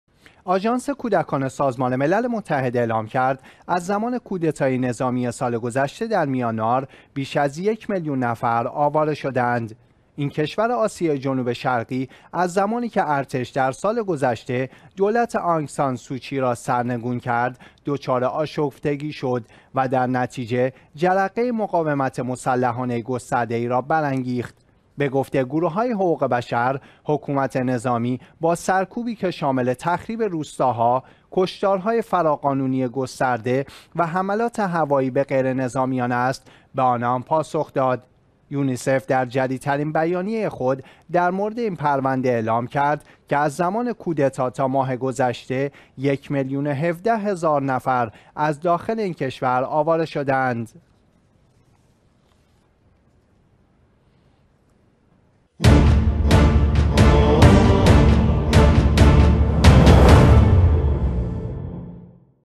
سازمان ملل: آواره شدن بیش از یک میلیون نفر پس از کودتای میانمار
آژانس کودکان سازمان ملل متحد اعلام کرد: از زمان کودتای نظامی سال گذشته در میانمار بیش از یک میلیون نفر آواره شده اند.

این کشورِ آسیای جنوب شرقی، از زمانی که ارتش در سال گذشته، دولت «آنگ سان سوچی» را سرنگون کرد، دچار آشفتگی شد و در نتيجه جرقه مقاومت مسلحانه گسترده ای را برانگیخت.

به گفته گروه های حقوق بشر، حکومتِ نظامی، با سرکوبی که شامل تخریب روستاها، کشتارهای فراقانونی گسترده و حملات هوایی به غیر نظامیان است، به آنان پاسخ داد.

یونیسف در جدید ترین بیانیه خود در مورد این پرونده، اعلام کرد که از زمان کودتا تا ماه گذشته، یک میلیون و ۱۷ هزار نفر داخل این  کشور آواره شده اند.